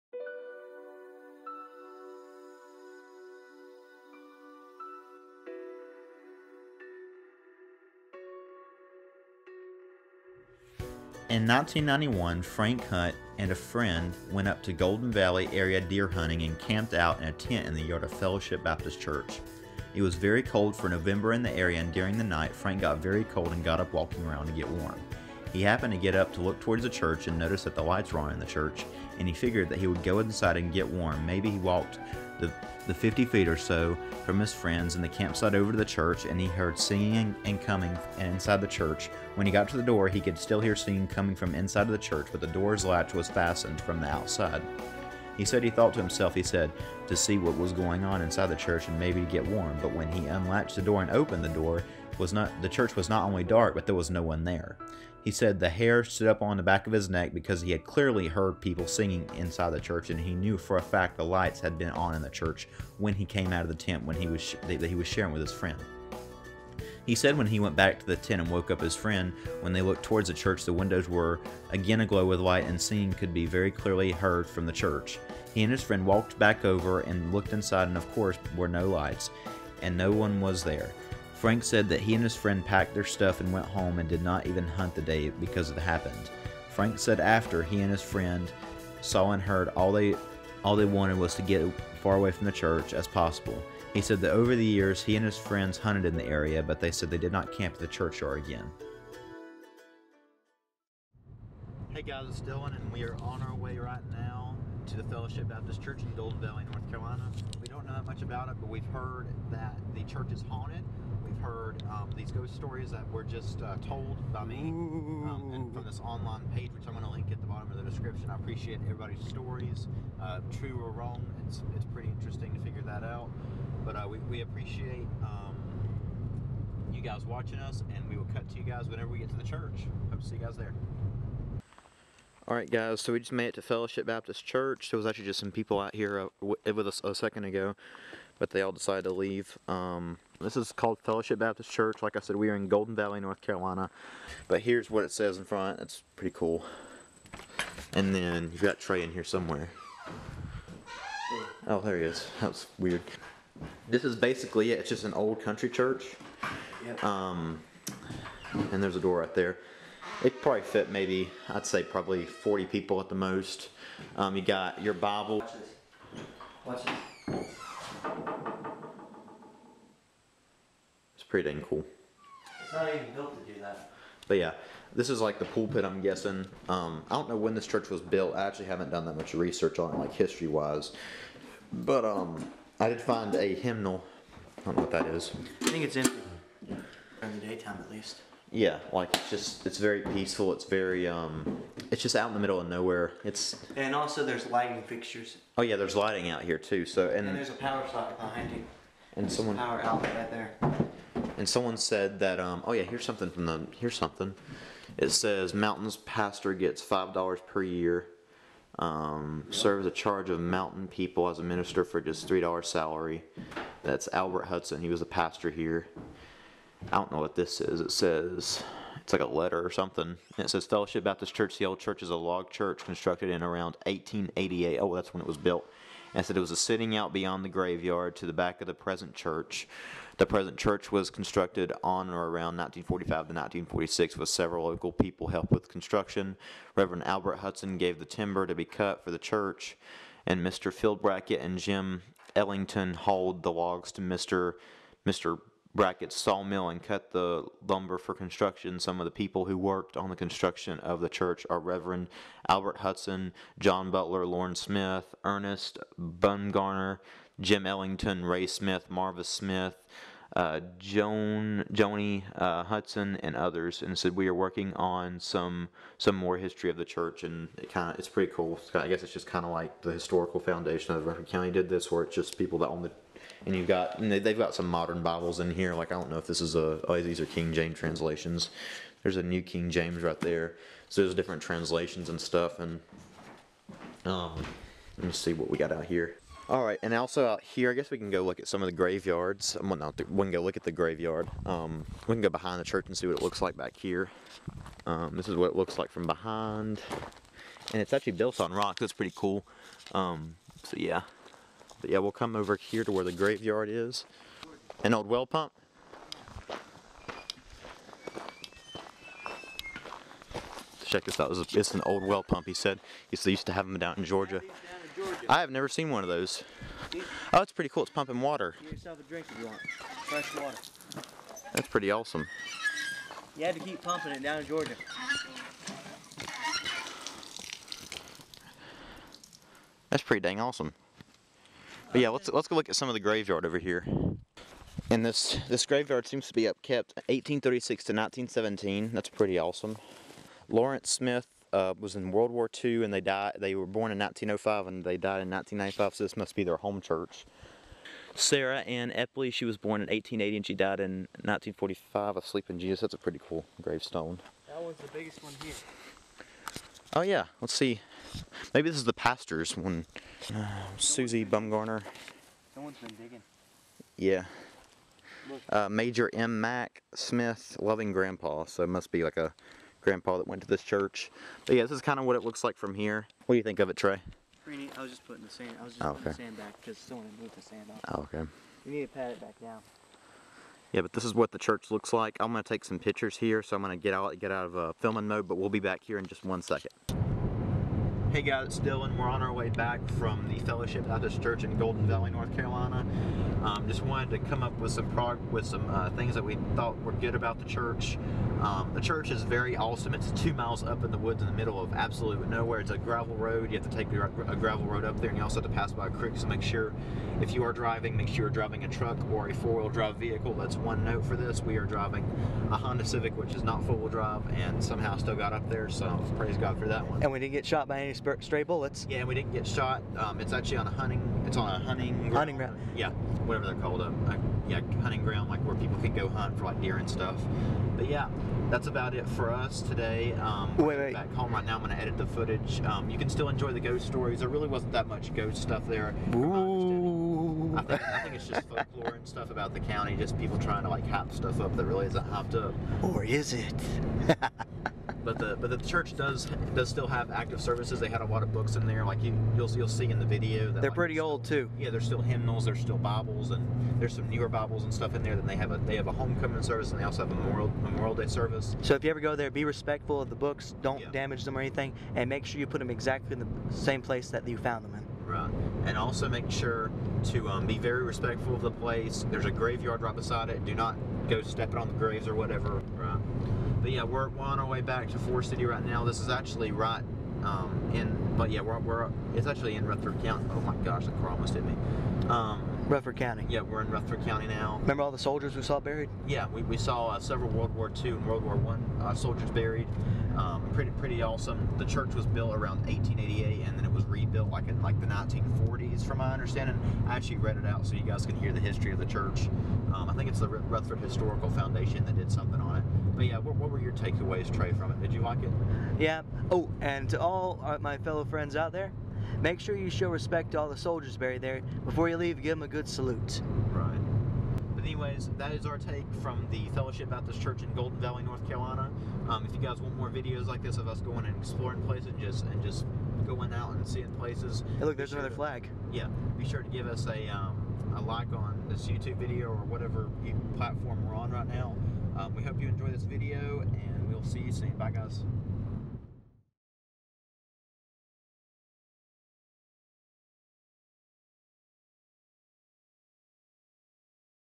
Fellowship Baptist Church Tour & Ghost Stories - Golden Valley, North Carolina
Fellowship Baptist Church was built around 1888 and it is in the mountains of Western North Carolina. The small church has a rich history in Rutherford County the small church has been a beacon for several generations. In the video you will see what the church looks like and you will also get to hear some of the history behind the small church. From our experience at the church we believe there is no paranormal things at the church because of the great environment and the loving atmosphere.

The church is located in Western North Carolina in a small town called Golden Valley. It is a 2 mile drive up a gravel road to reach the church. While on the way to the church you cross a creek and we do recommend you either drive a truck or a 4WD vehicle to the church.

My name is Dylan Henson and I am majoring in Broadcasting and Audio Production. Me and my friend Trey Lawson decided to start a mini-documentary series about historical places in our area.

The Fellowship Baptist Church is an awesome place to visit and we do ask that you be respectful of any and all property. Thanks!